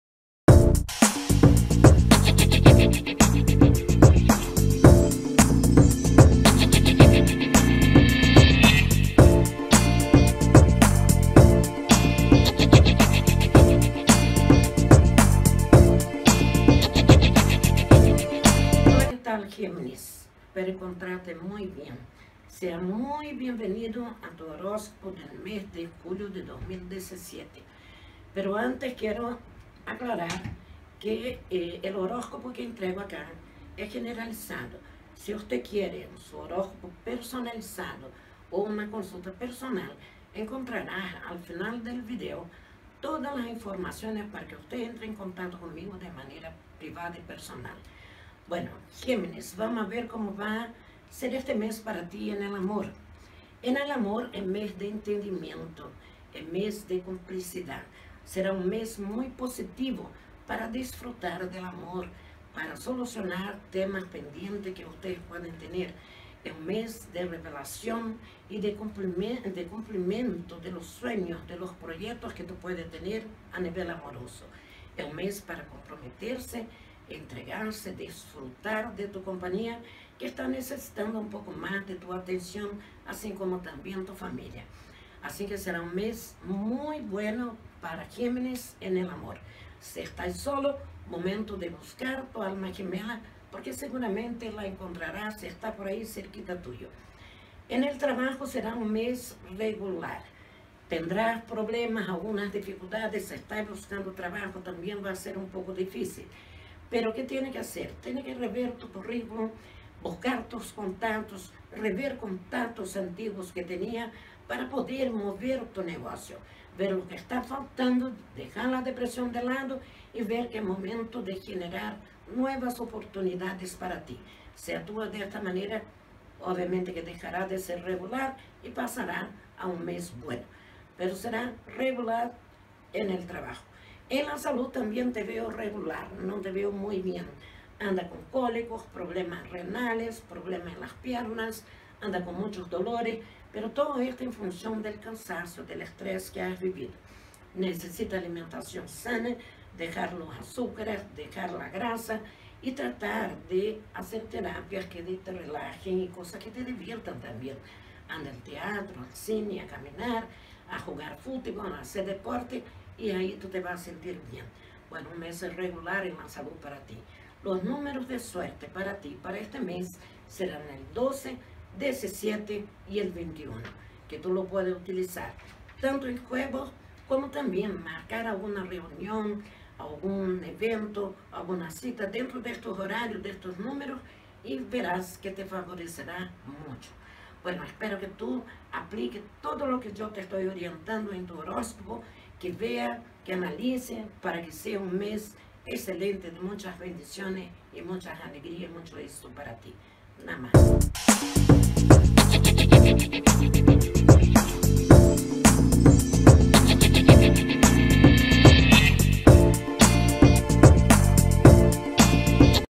GEMINIS JULIO 2017
En este vídeo entregare el horóscopo de Julio para el signo de Geminis, hablare de amor, salud y trabajo. Entregando tips para que les valla super bien y puedan tener un excelente mes!!
¡Namaste!
Aplíquenlo!!!

Fatima Gomes Copyright ®.